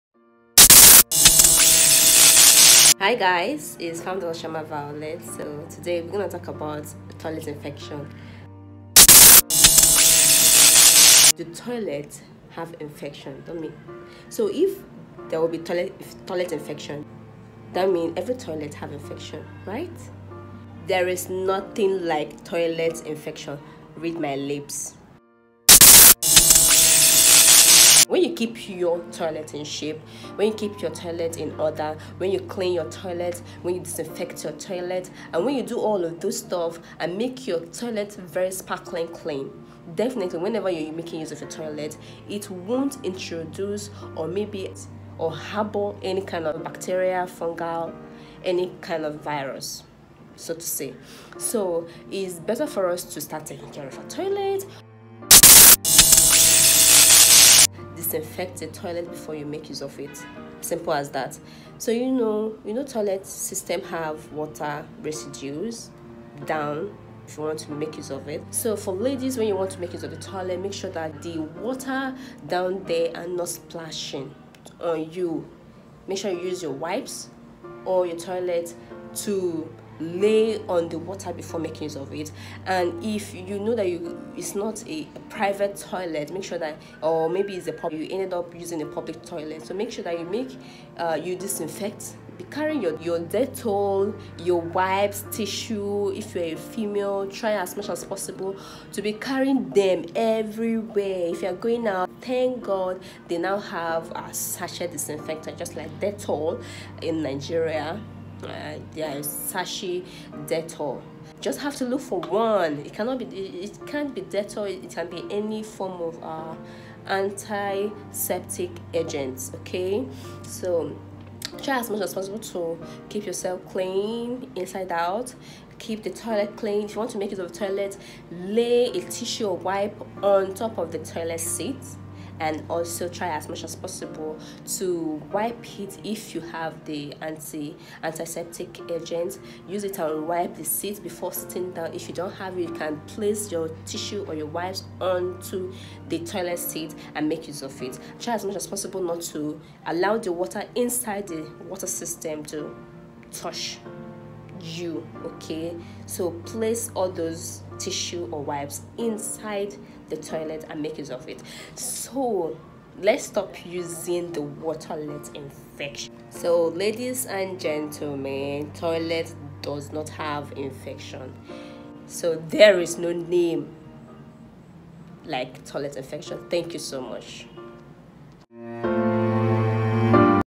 About Toilet 🚽 Infections; Things you need to know
I CAN’T KEEP CALM 🤸🏽‍♀️🤸🏽‍♀️🤸🏽‍♀️🤸🏽‍♀️🤸🏽‍♀️🤸🏽‍♀️🤸🏽‍♀️🤸🏽‍♀️🤸🏽‍♀️🤭🤭🤭🤗🤗🤗

Now the Long awaited is here guys ..

Again the BIG QUESTION IS

 “TOILET INFECTION; A MYTH OR FACT?”

 “DO TOILET HAVE INFECTIONS!?”

Okay, now a lot of Ladies do complain about toilet infections and all and all and all, so it’ll be so nice if you see this video and empower yourself through it🥱, the knowledge and wisdom that lies innit 🤗!
Say no to procrastination and see it now
Pls do well to share it (very necessary guys, don’t learn alone 🥱)
WITH ALL LOVE AND EFFORTS TO MAKE THIS HAPPEN, we are so pleased to announce, we have make half impact in your life , yeeeeeeeee🤸🏽‍♀️🤸🏽‍♀️🤸🏽‍♀️🤸🏽‍♀️🤸🏽‍♀️🤸🏽‍♀️
Also tell me what you think 🤔?